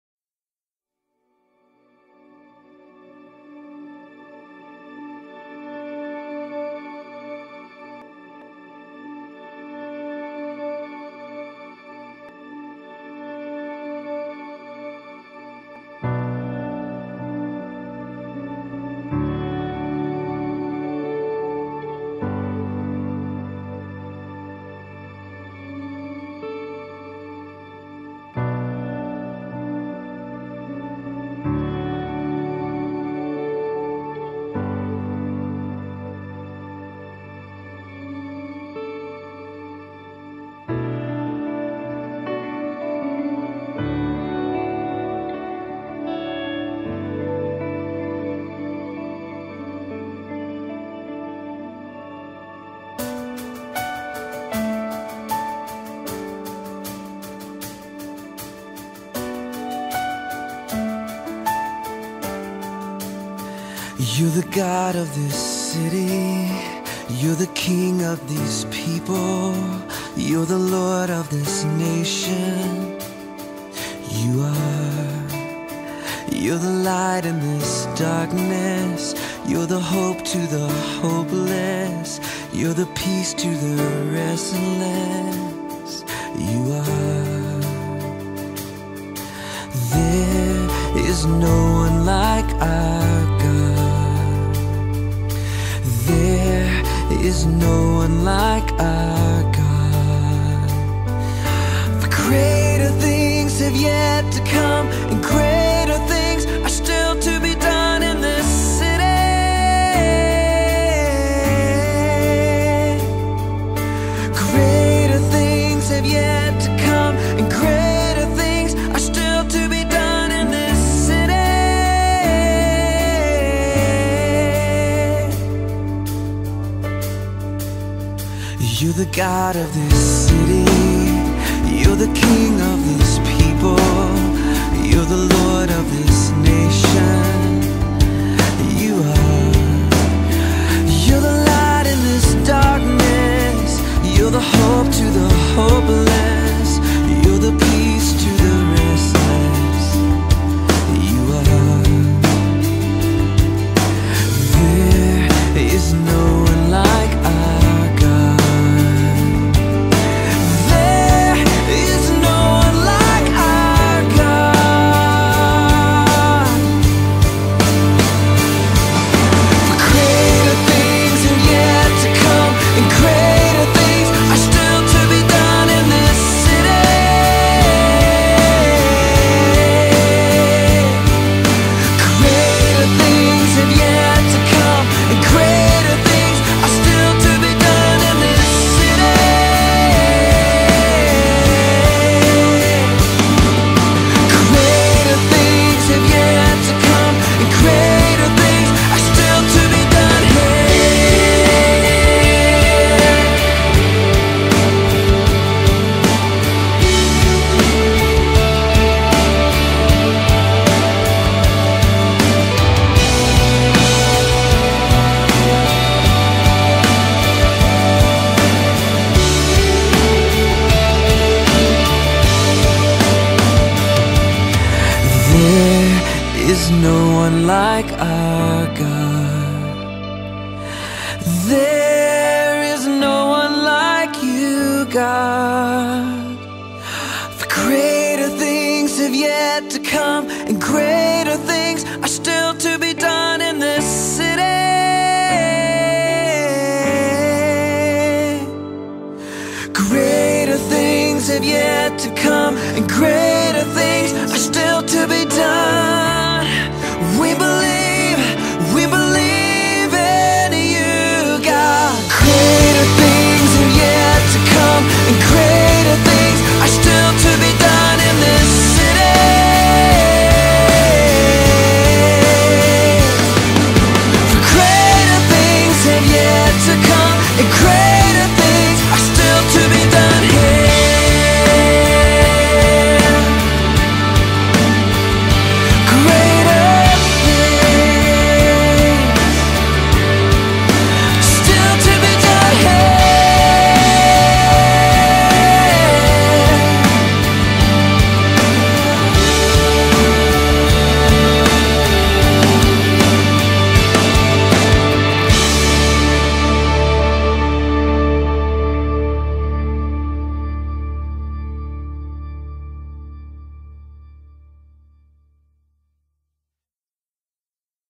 In This City (Millen Ga)
Video for Thanksgiving Rally Monday Nov 22, 2010.   Church Pics by Tim & Kelly Fields